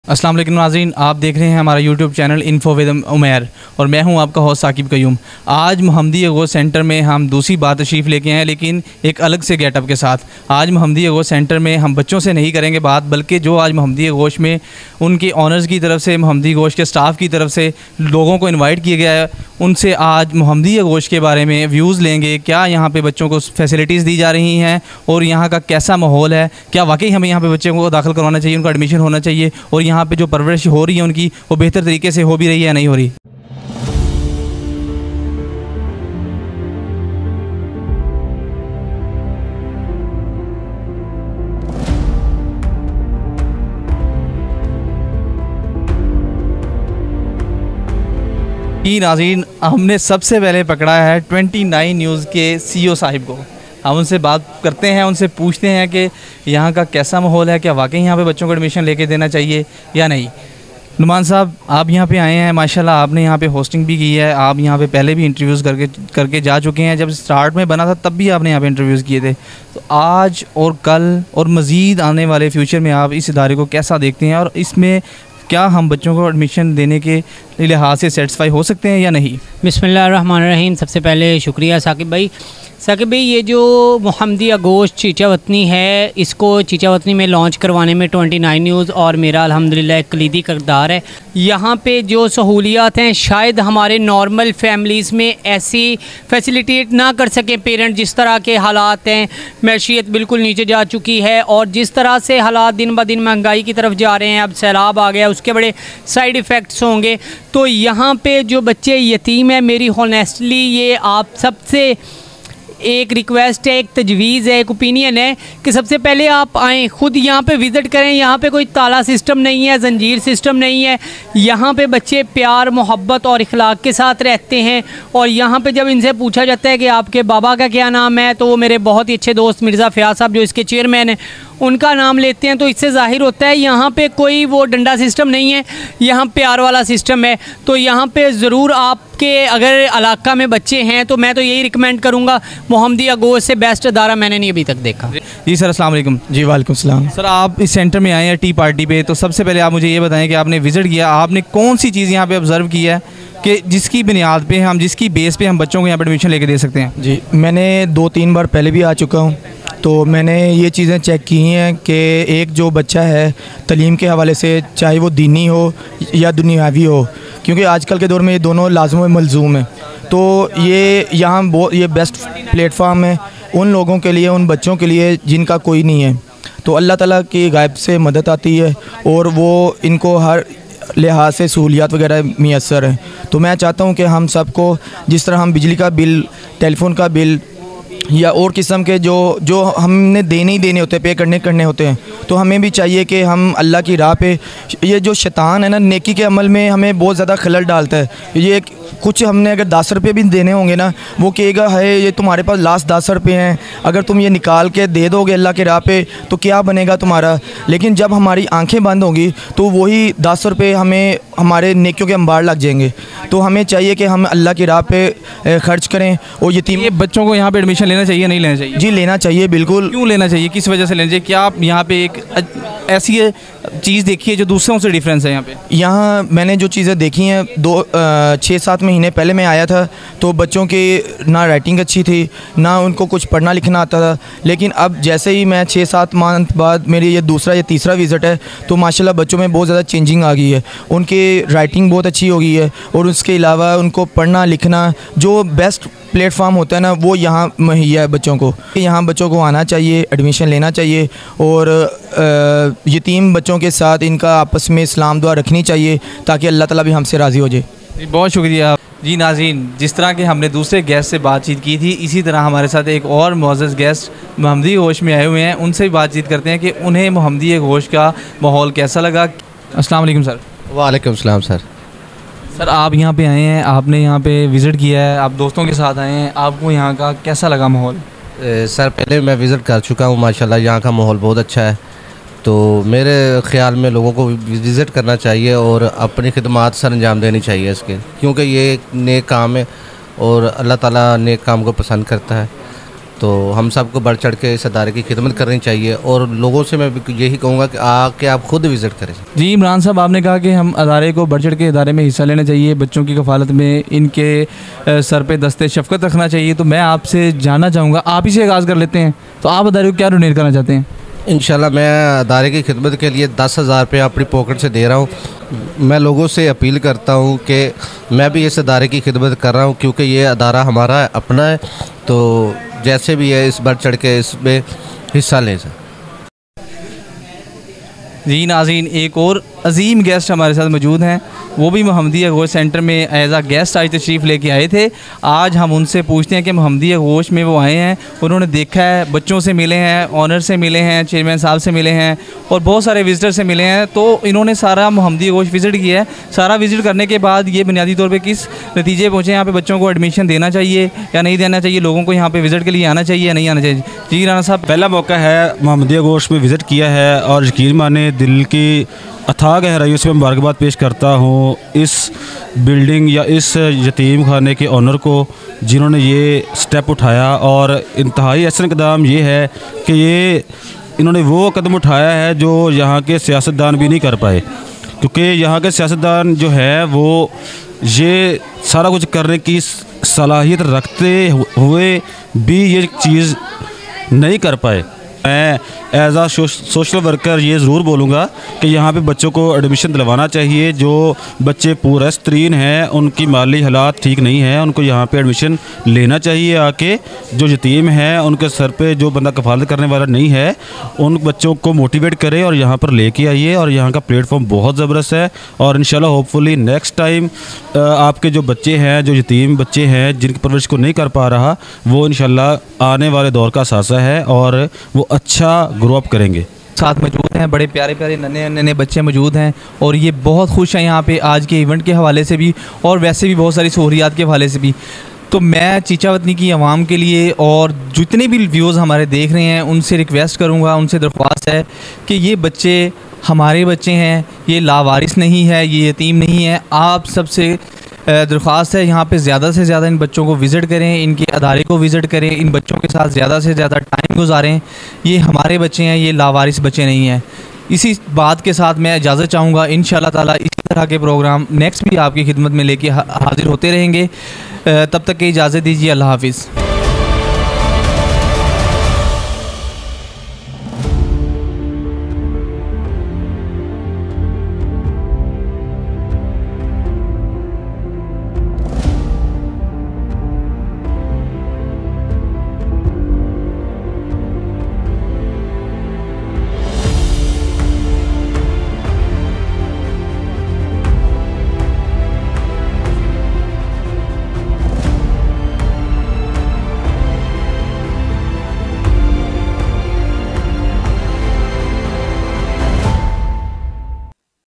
People's views about Muhammadi Agosh Foundation again Visit
Assalam.o.Alaikum
In today's video, we re-visited the foundation and we got feedback from the guests coming there.
For more information about the foundation, please contact this number..

Chairman Muhammad Fayyaz : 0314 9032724